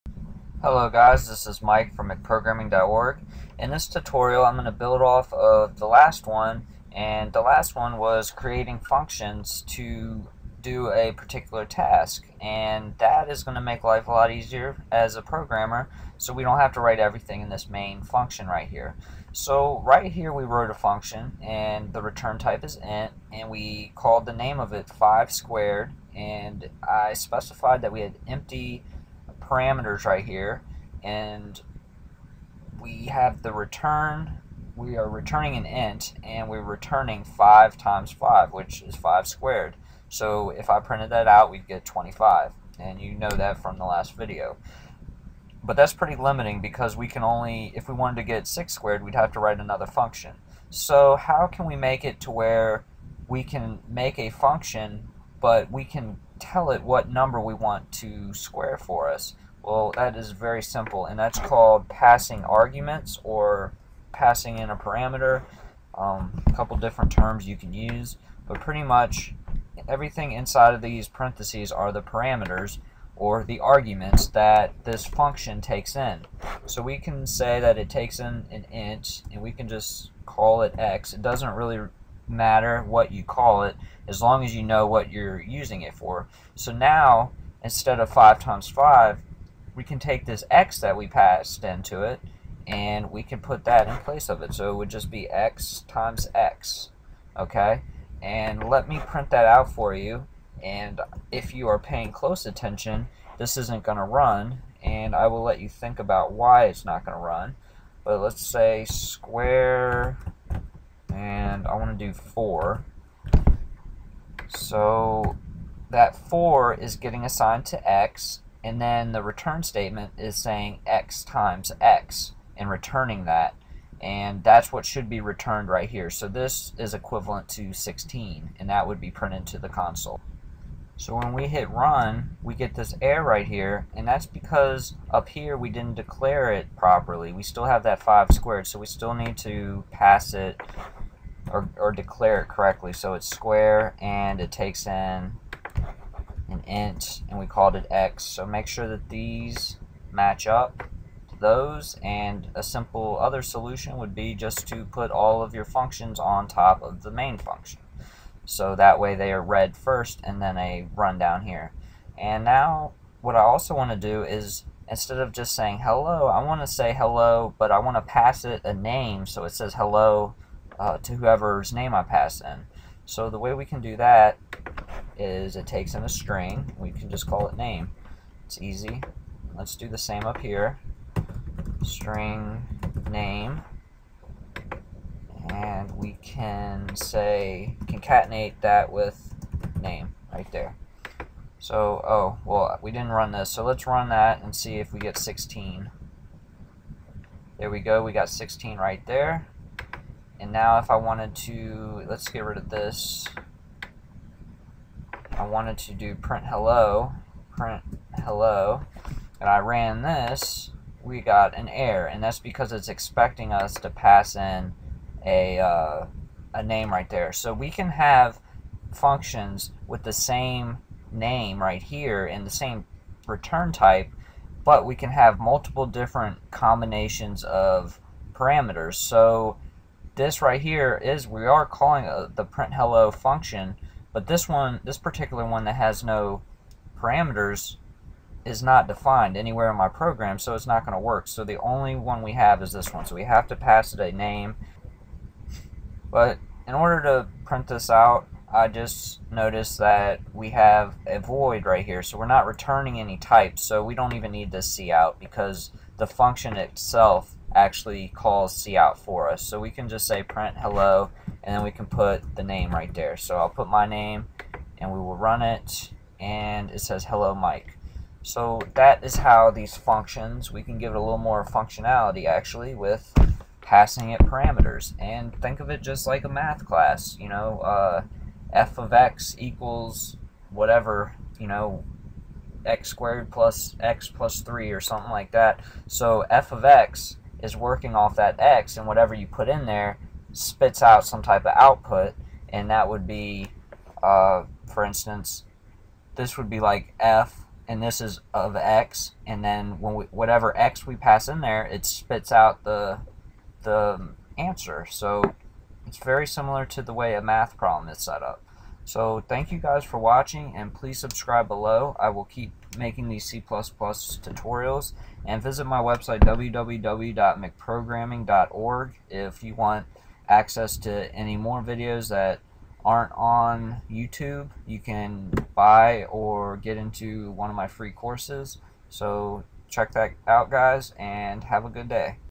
C++ - Functions with Parameters (Arguments)
C++ functions with parameters (arguments) - In this C++ video I show you how make c++ programs more efficient by passing in arguments to a functions parameters to perform the specific task on the argument passed in.